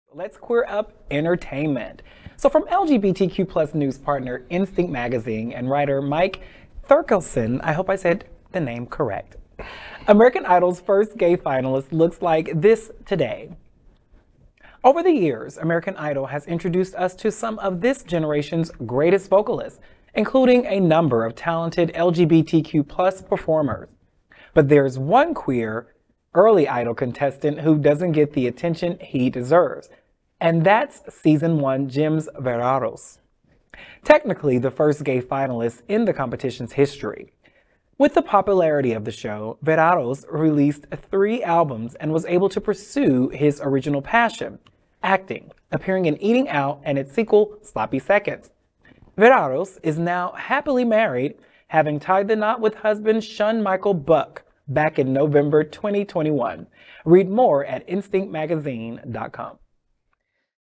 American Idol's First Gay Finalist Looks Like This Today
Over the years, American Idol has introduced us to some of this generation’s greatest vocalists, including a number of talented LGBTQ+ performers. But there’s one queer, early Idol contestant who doesn’t get the attention he deserves, and that’s season one’s Jim Verraros—technically the first gay finalist in the competition’s history.
With the popularity of the show, Verraros released three albums and was able to pursue his original passion, acting, appearing in Eating Out and its sequel Sloppy Seconds.